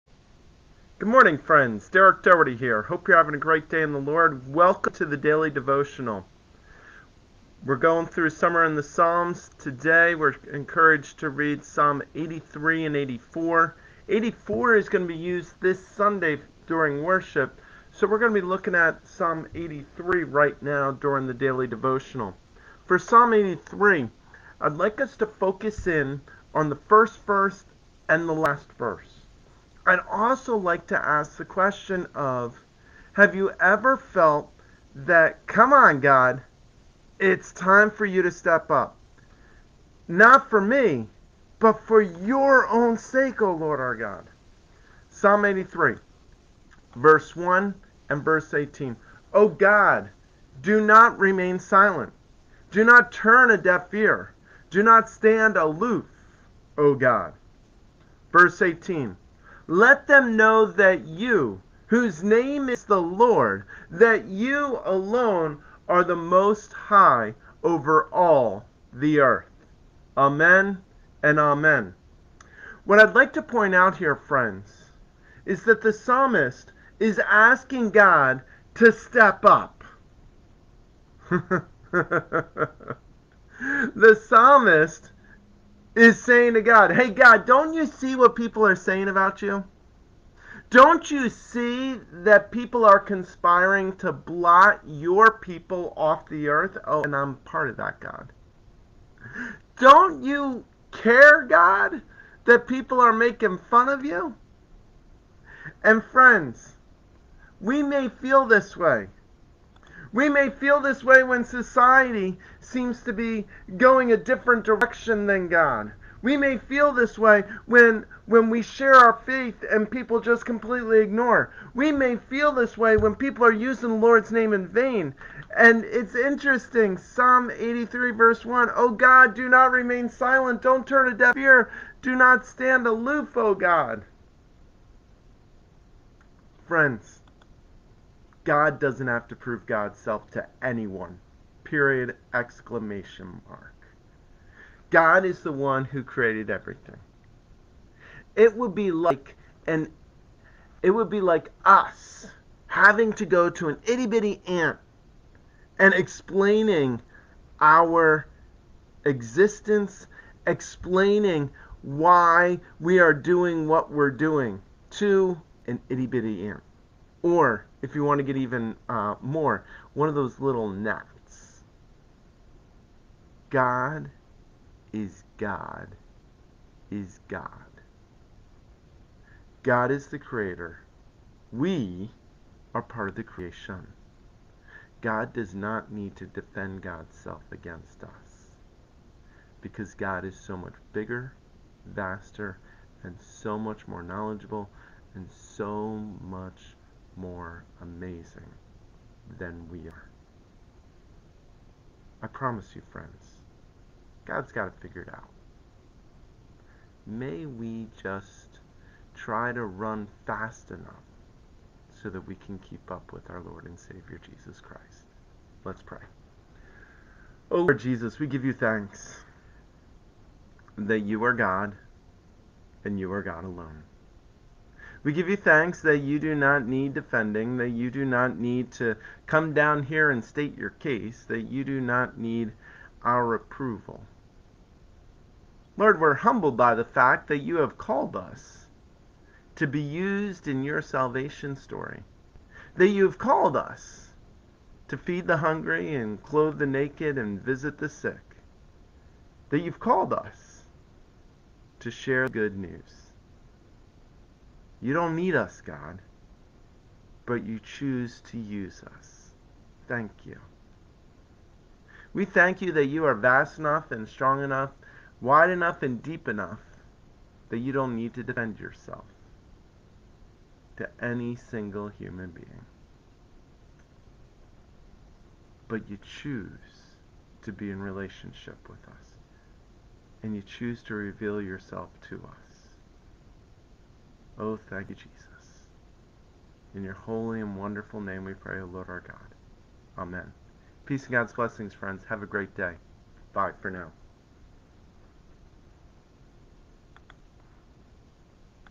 Daily Devotional (Psalm 83)
Good Morning
Summer in the Psalms
Psalm 83.1+18
O God, do not remain silent; do not turn a deaf ear, do not stand aloof, O God.
Let them know that you, whose name is the Lord—that you alone are the Most High over all the earth.